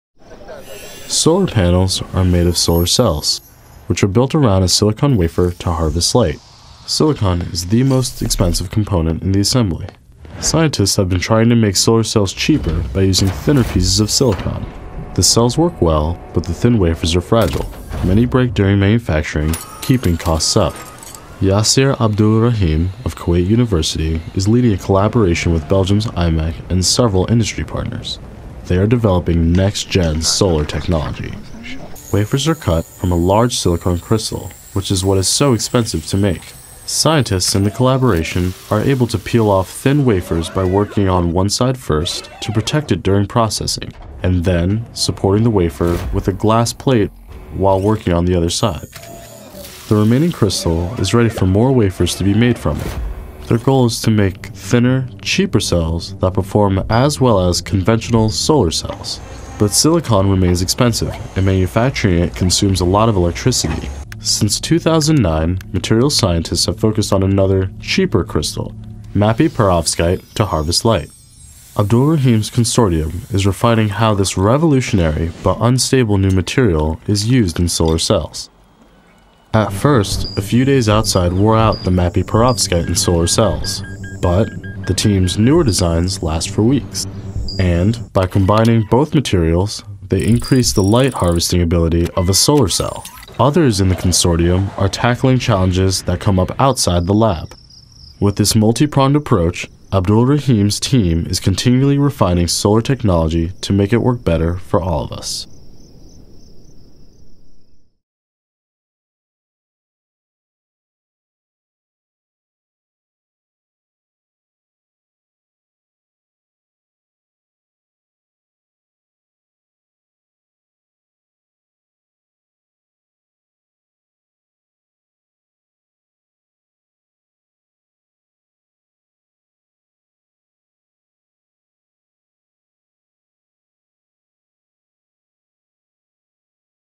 Advertisement Feature from KFAS: Slimmer solar cells
Solar cells could be made cheaper by using less silicon, which is the most expensive part of the assembly. This animation describes how Yaser Aldulraheem’s team collaborated with IMEC to make cheaper solar cells by using thinner silicon wafers and alternative materials. Produced for the Kuwaiti Foundation for the Advancement of Sciences (KFAS) by Nature Research Custom Media, which operates independently of other Nature Research editorial departments.